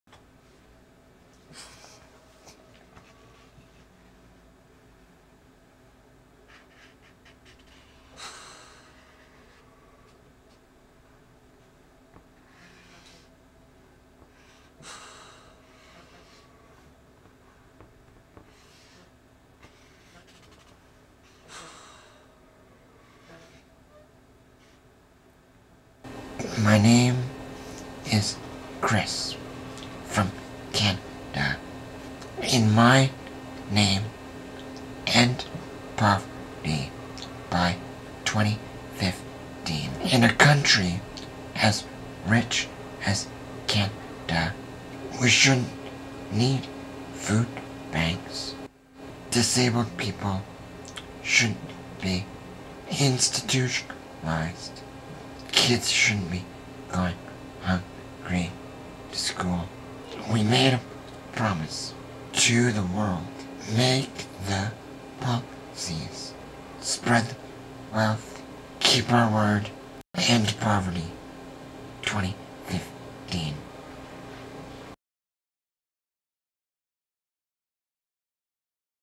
Chris, Canada, in my name
Chris from Canada, in my name end poverty by 2015. Get on it governments of the world.